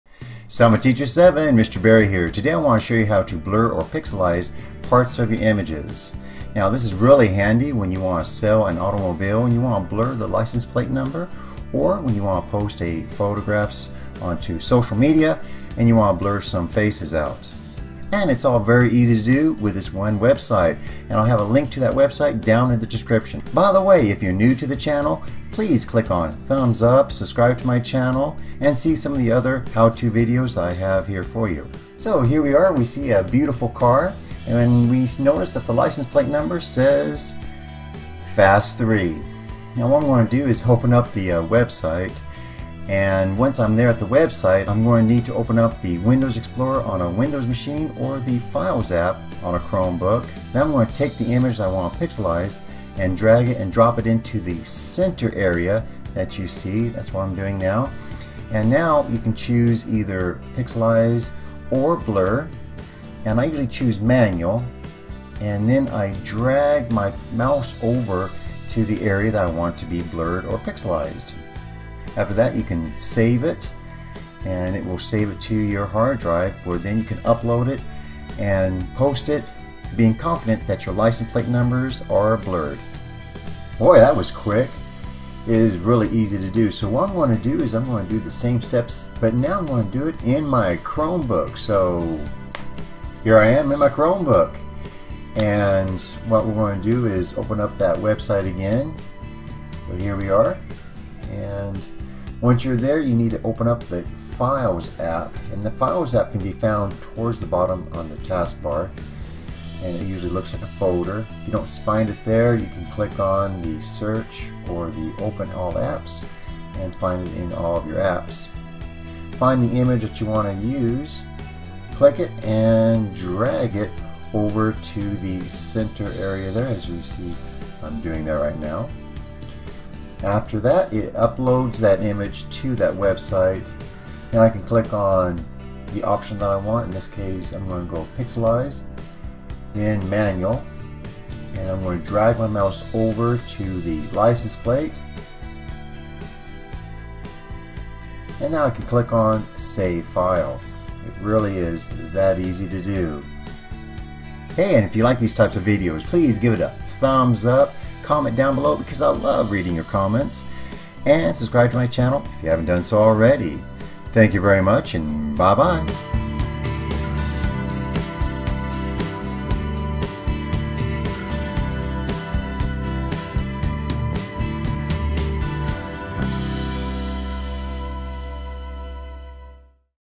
How to Blur a Face or Licence Plate Number on Photos without Photoshop
CAMERA/LENS:
All of the outside shots:

LIGHTING:
Selfie Ring Camera Light.

SOFTWARE:
VideoPad professional by NCH
L-View Pro
Chrome OS
Windows 10
Screencastify